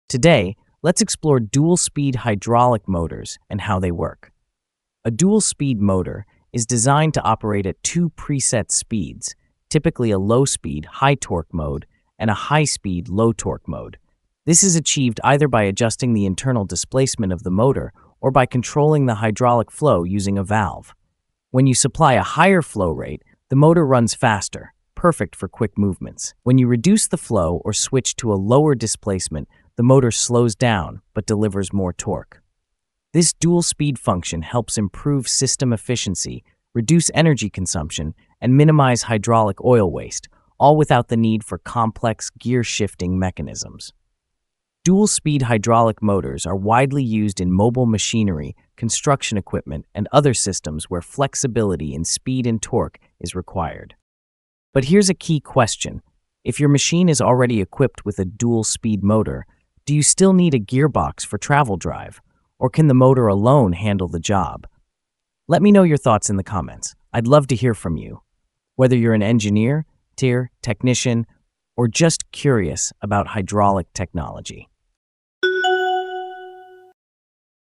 How Dual-Speed Motors Work — And Do You Still Need a Gearbox?
Dual-speed motors offer two preset operating speeds — usually high and low — by adjusting flow or displacement. In hydraulic systems, they’re commonly used to improve efficiency and reduce energy waste. But here’s the big question: if your drive system already has a dual-speed hydraulic motor, do you still need a gearbox?